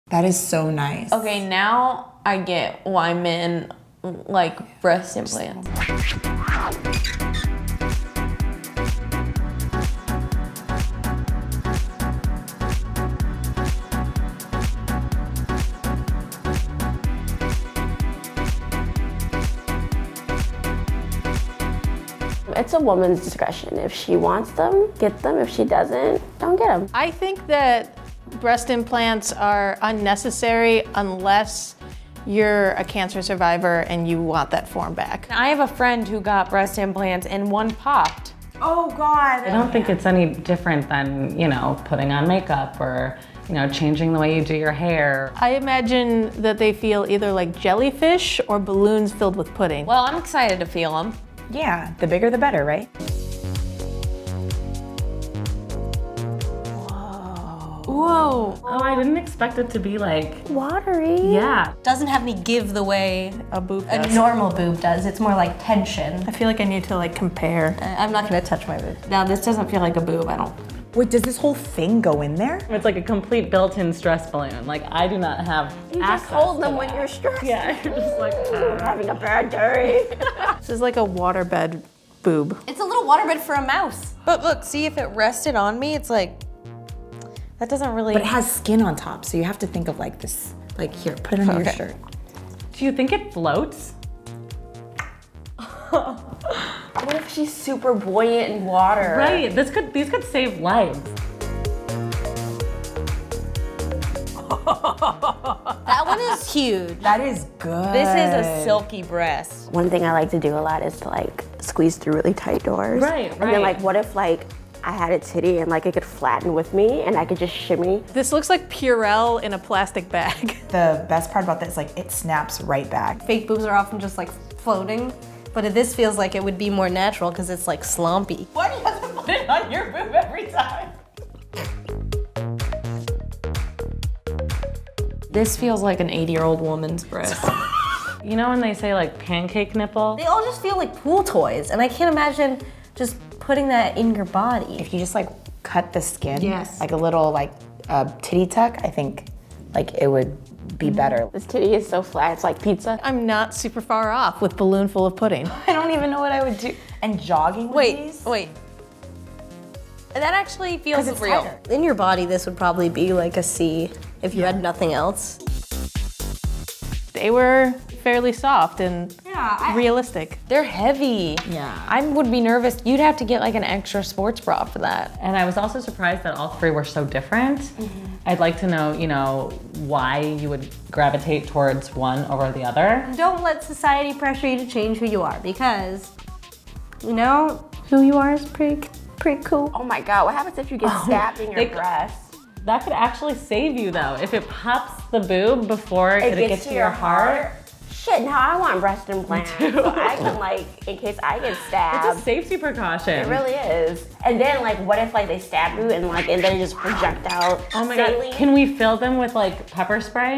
Women Feel Breast Implants For The First Time
“This is a silky breast.”

MUSIC
Hammond Eggs

SPECIAL THANKS TO:
Dr. Andrew Cohen
Shelley Hirschman